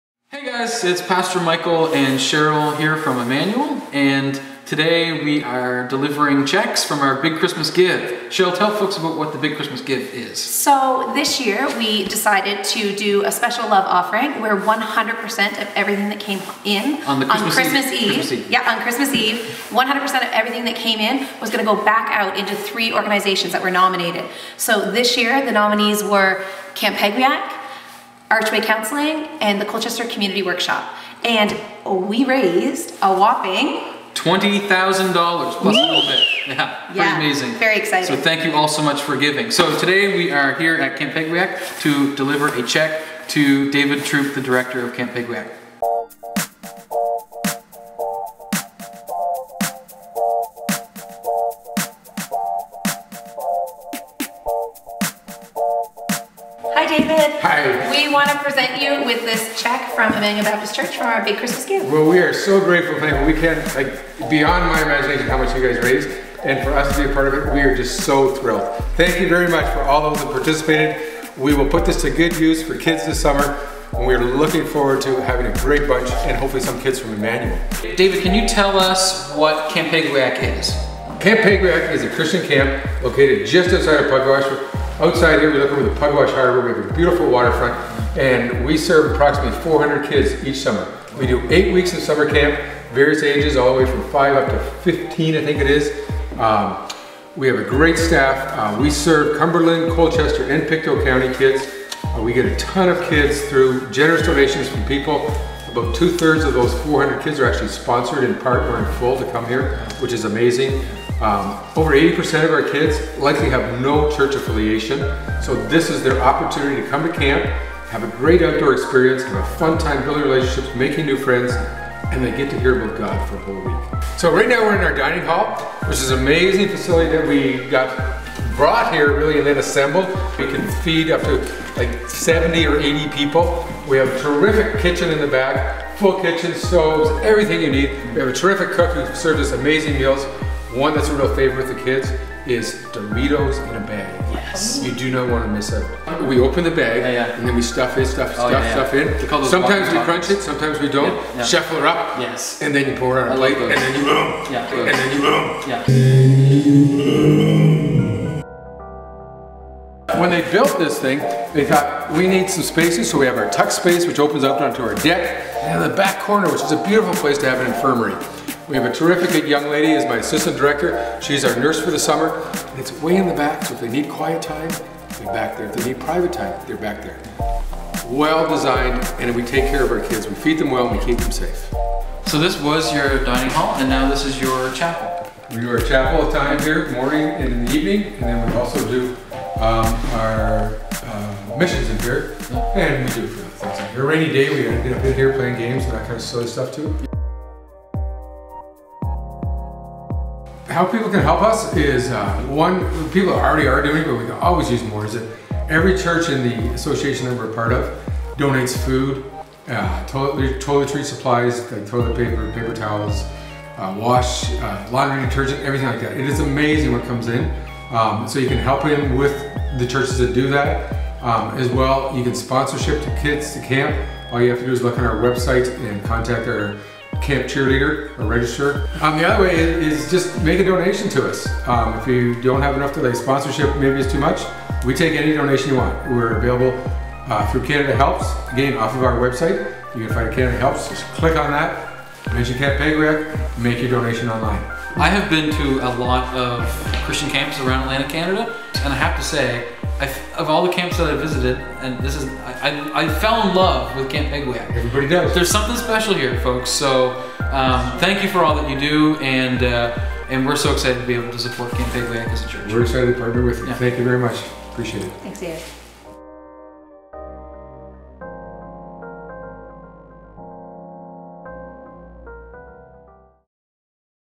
Big Christmas Give 2019 Camp Pagweak
Visiting Camp Pagweak with Director David Throop